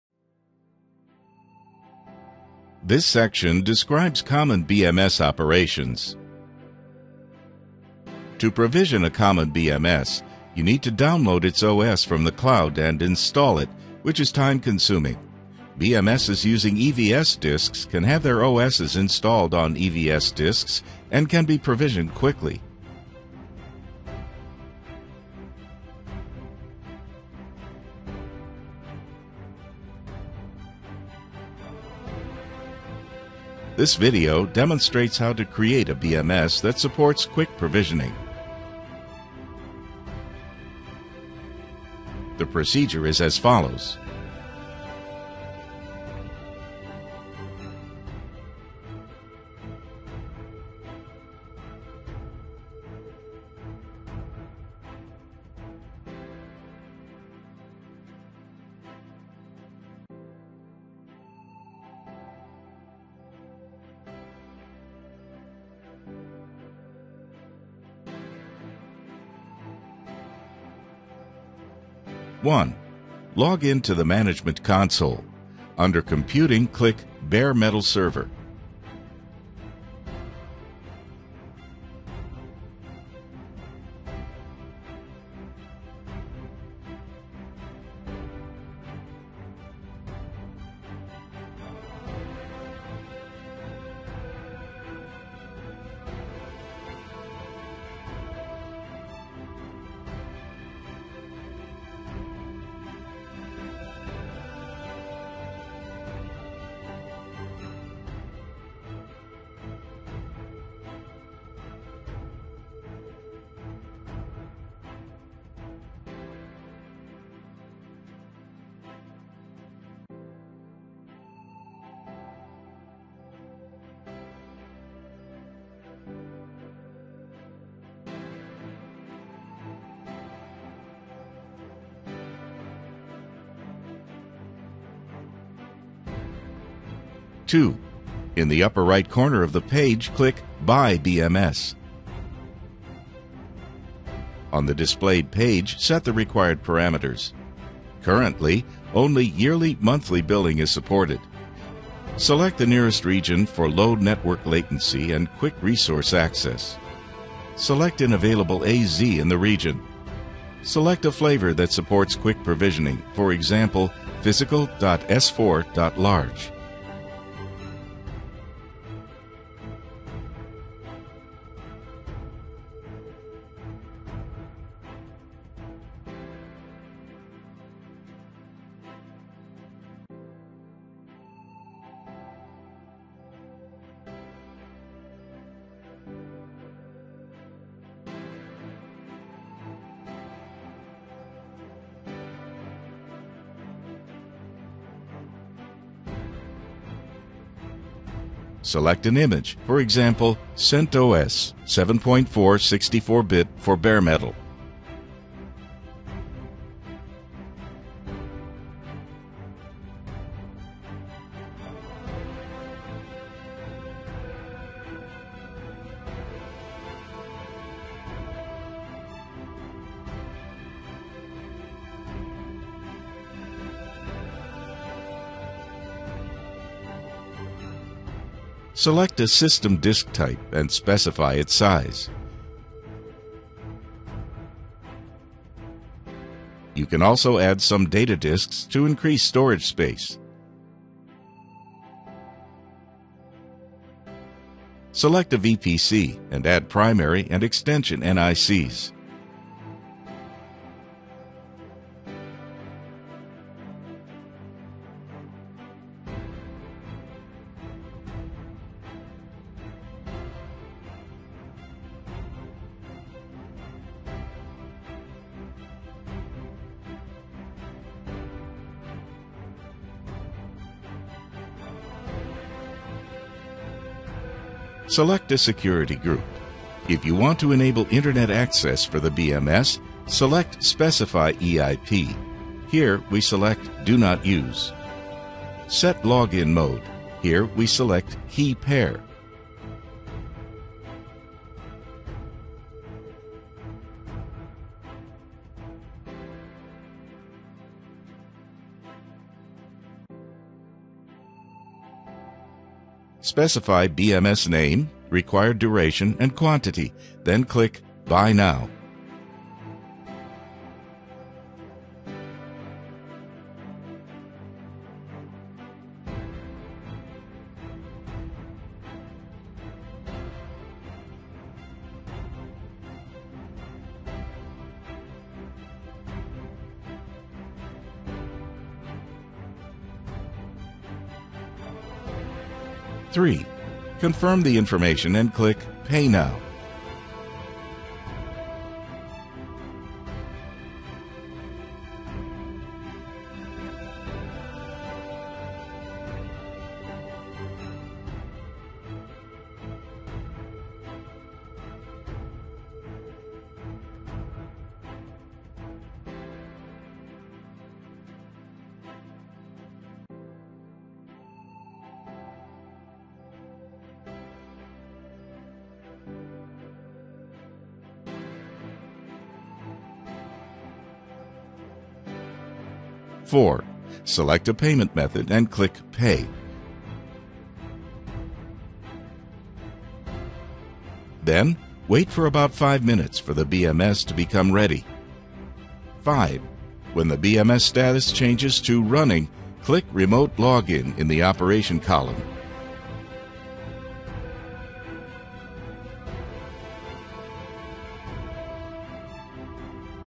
HUAWEI CLOUD Bare Metal Server (BMS): Creating a BMS Supporting Quick Provisioning
This video will describe the common BMS operations with application scenarios, procedure and step-by-step guidance to create a BMS that supports quick provisioning.